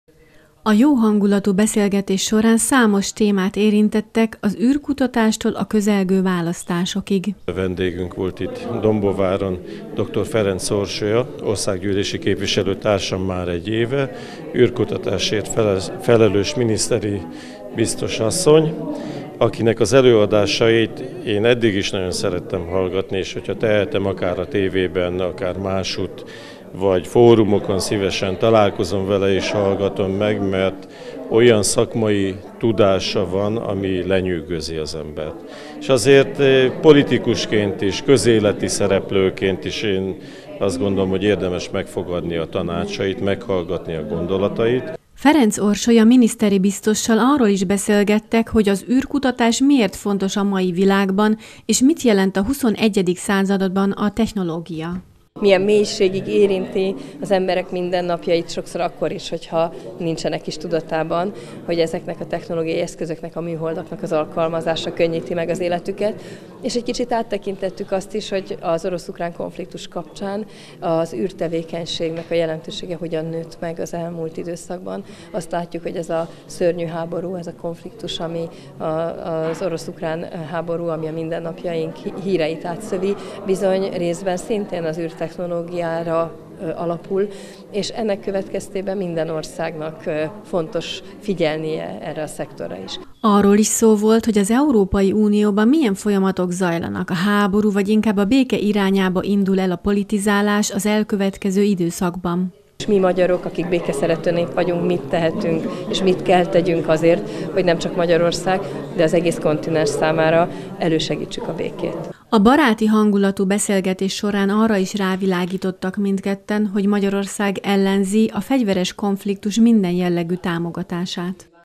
Dombóvári fórum - űrkutatástól a közelgő választásokig
Dr. Ferencz Orsolya miniszteri biztos, parlamenti képviselő volt vendége Dombóváron Pintér Szilárd polgármesternek és Potápi Árpád János államtitkár, országgyűlési képviselőnek.